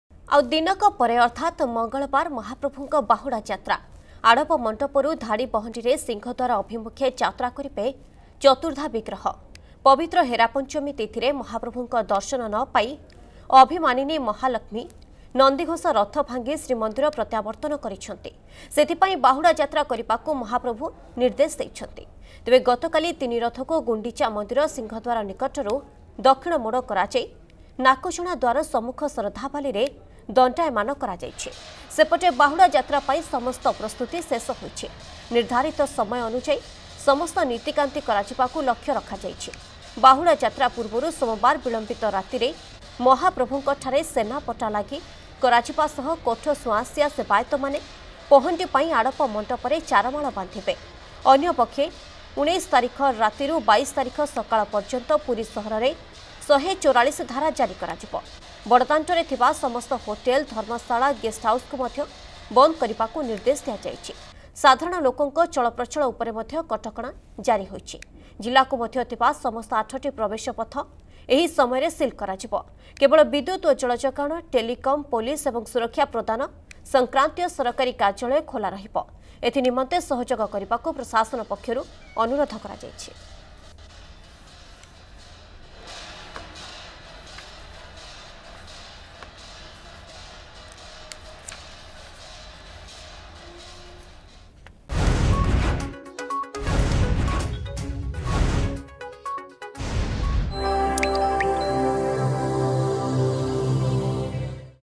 ମଙ୍ଗଳବାର ମହାପ୍ରଭୁଙ୍କ ବାହୁଡାଯାତ୍ରା
ଆଉ ଦିନକ ପରେ ଅର୍ଥାତ ମଙ୍ଗଳବାର ମହାପ୍ରଭୁଙ୍କ ବାହୁଡାଯାତ୍ରା। ଆଡପମଣ୍ଡପରୁ ଧାଡି ପହଣ୍ଡିରେ ସିଂହଦ୍ଵାର ଅଭିମୁଖେ ଯାତ୍ରା କରିବେ ଚତୁର୍ଦ୍ଧାବିଗ୍ରହ।